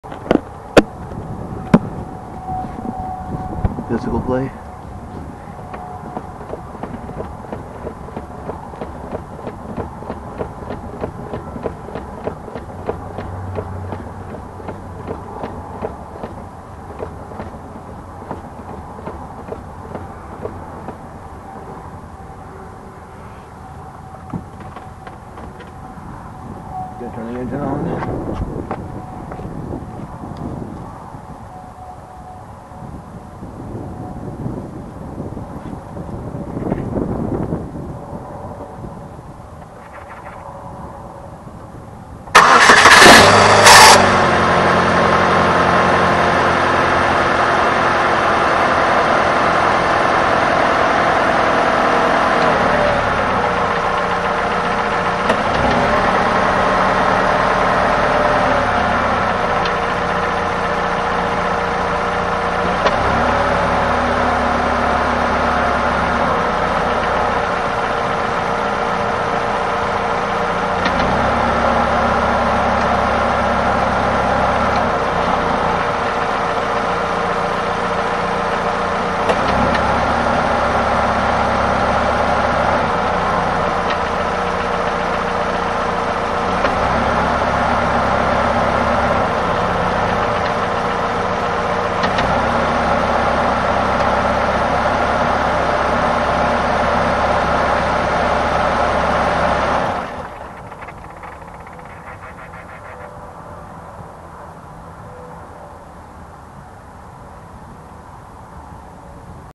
Bad engine mount? 03 cavalier 2.2 ecotec
Also, note the sound before starting, and after turning the car off, as I haven't been able to figure that out either...